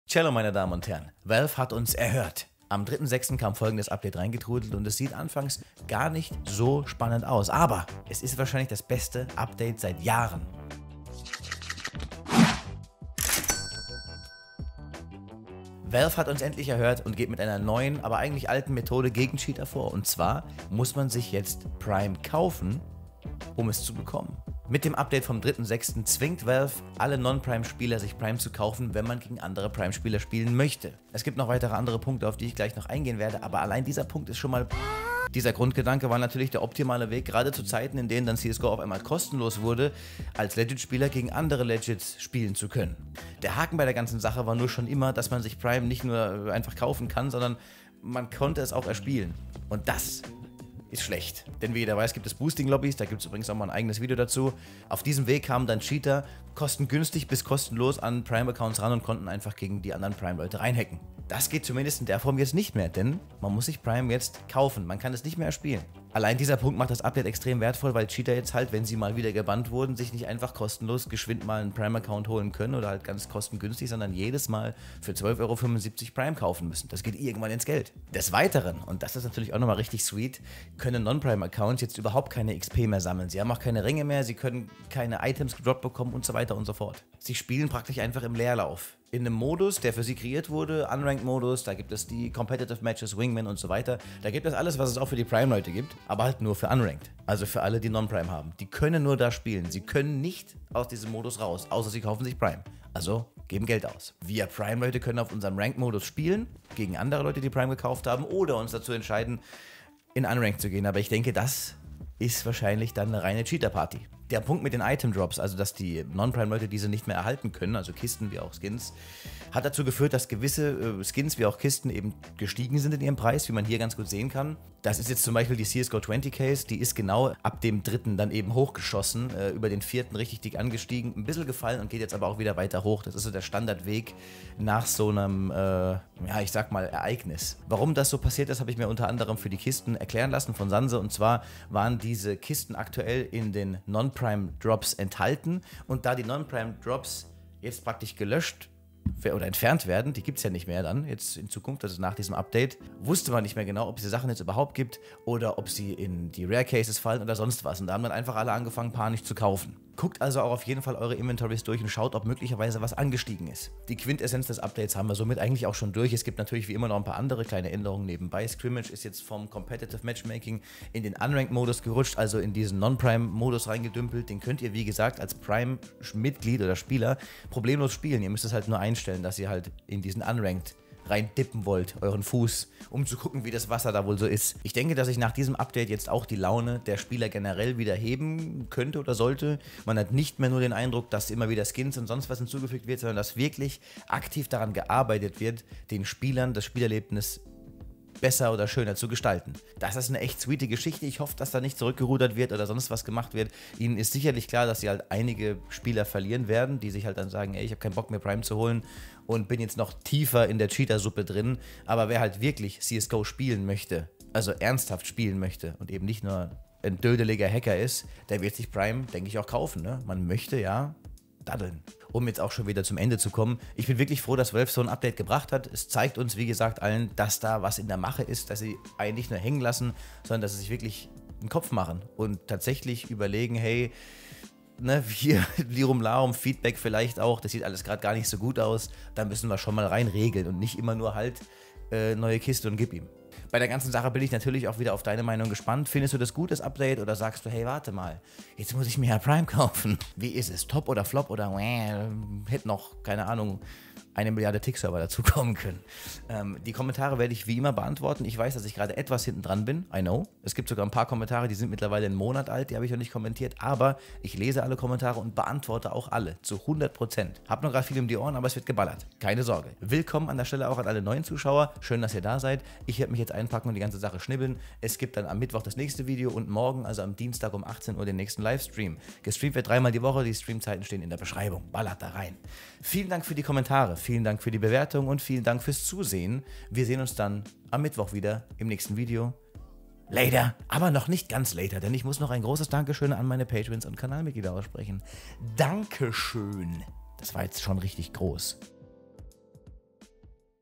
Valve geht aktiv gegen Cheater vor | GROẞES CS:GO UPDATE
Endlich ist es soweit! Valve knallt uns ein neues Update vor den Latz und es ist... echt extrem gut! Valve geht aktiv gegen Cheater vor, nebenbei bzw. durch das Update steigen auch noch die Preise einiger Kisten und Skins an :D OPTIMAL!
Vielen Dank an Sanse für die Infos :*

LIVESTREAM, VIEWERGAMES & Skingiveaways jeden Di und Do ab 18 Uhr Uhr und jeden Freitag ab 22 Uhr!

• Mein Epic Creator Code: DERDUMMBABBLER